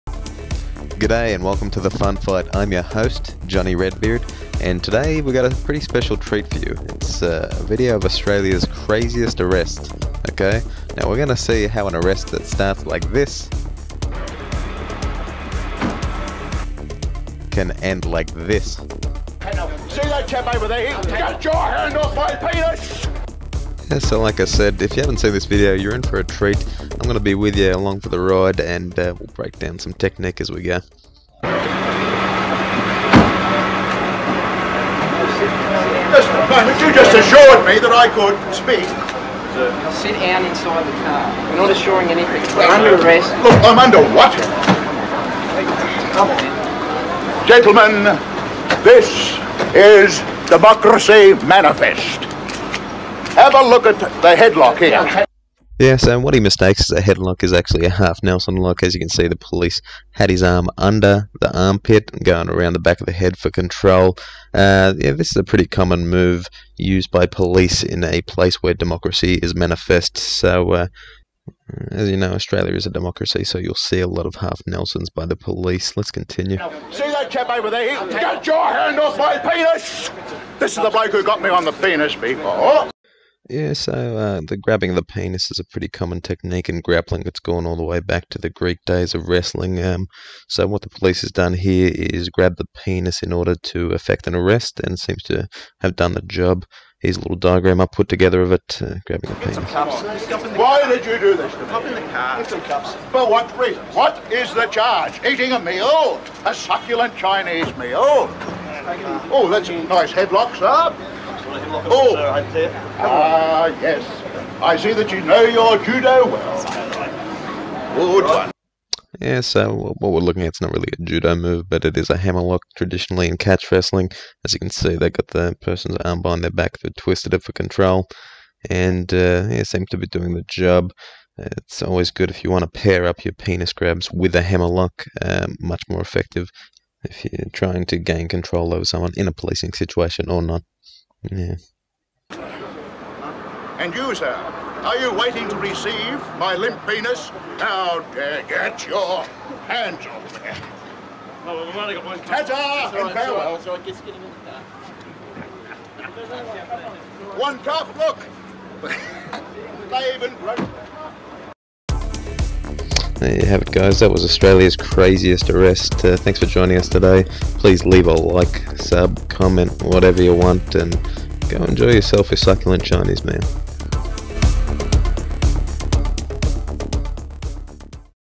INSANE Australian Arrest
Australia’s most infamous arrest by Queensland police.

This video covers the arrest that spawned famous sayings “I see you know your judo well” “this officer grabbed my penis” “is it a crime to enjoy a succulent Chinese meal”

We look at the techniques of the police in a satirical way.